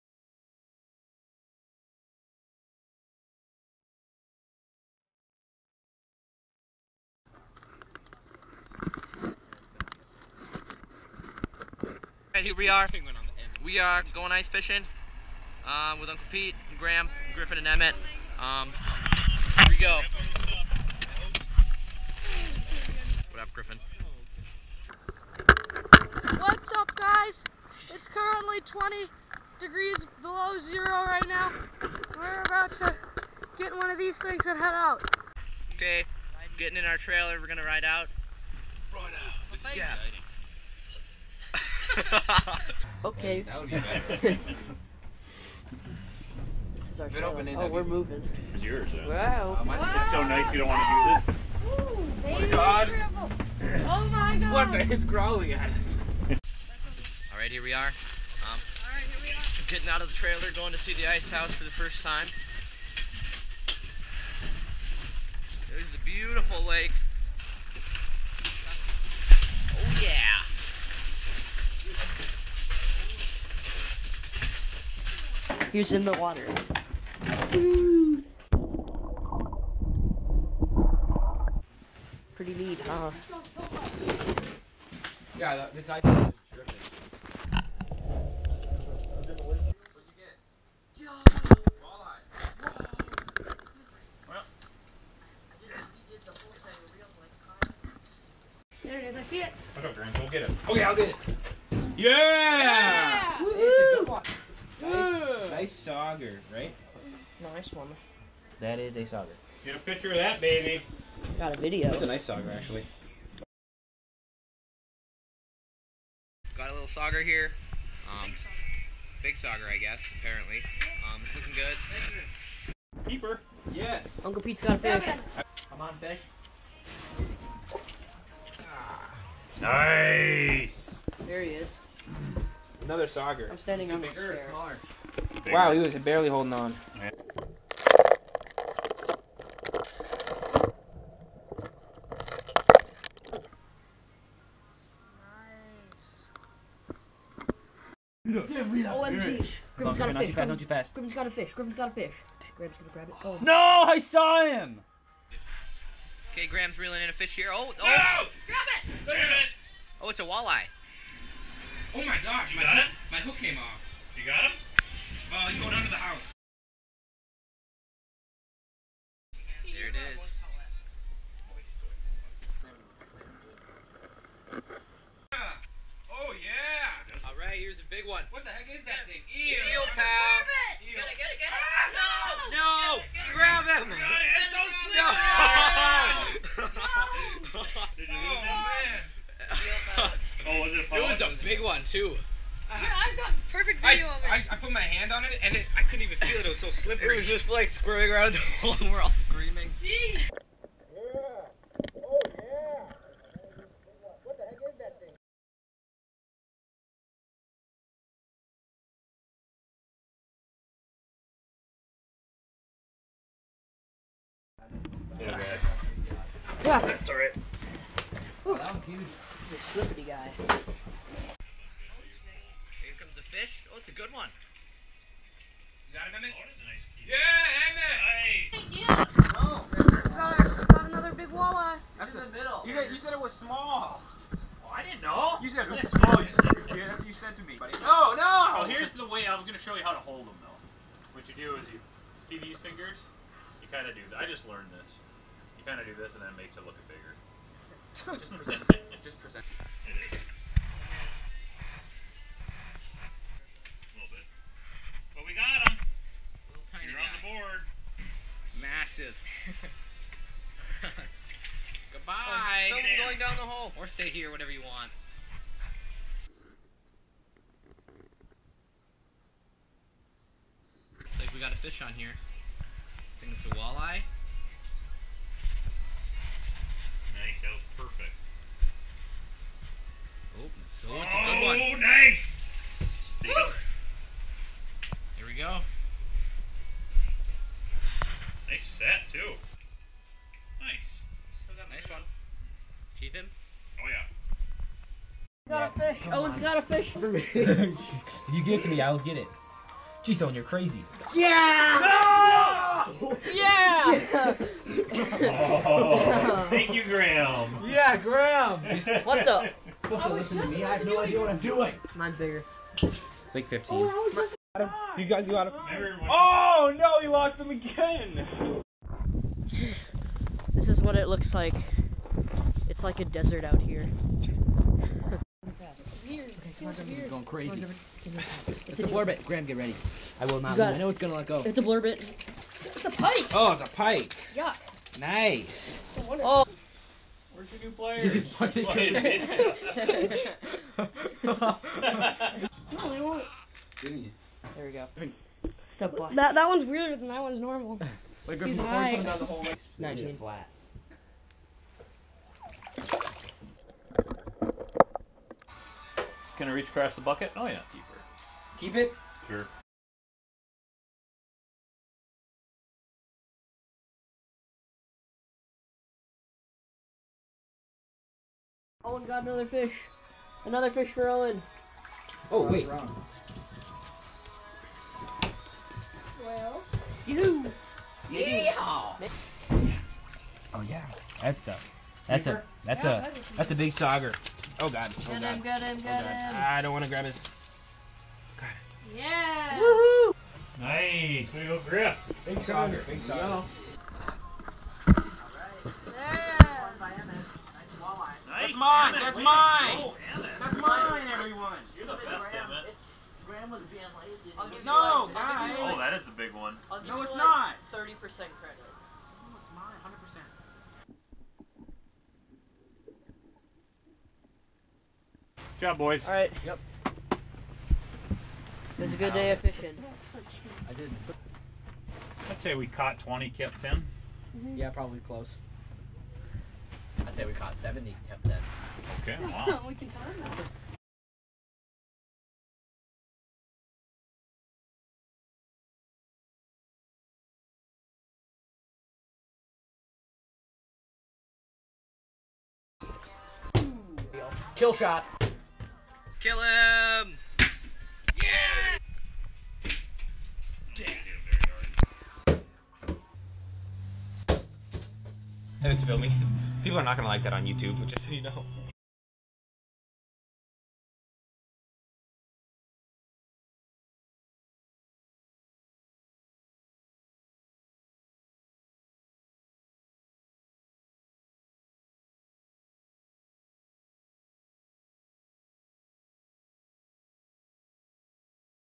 Ice Fishing on Lake of the Woods
December 2017 ice fishing on Lake of the Woods at Arneson's Rocky Point Resort in Roosevelt, MN.
Me (Owen), Emmett, Griffin, Graham, and my uncle Pete had a great time hanging out on the lake. We caught 20 fish and kept 10.